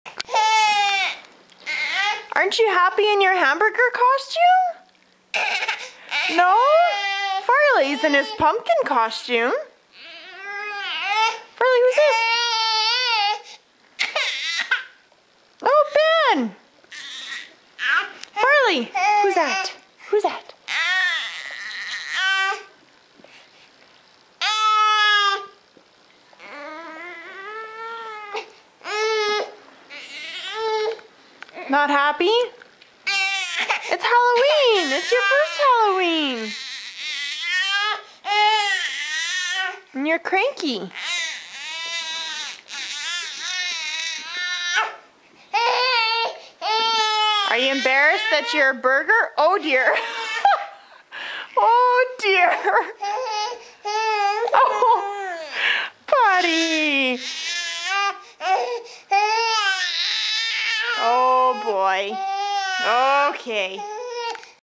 Ben's First Halloween
Dressed as a hamburger!  And pissed off about the crappy candy he got.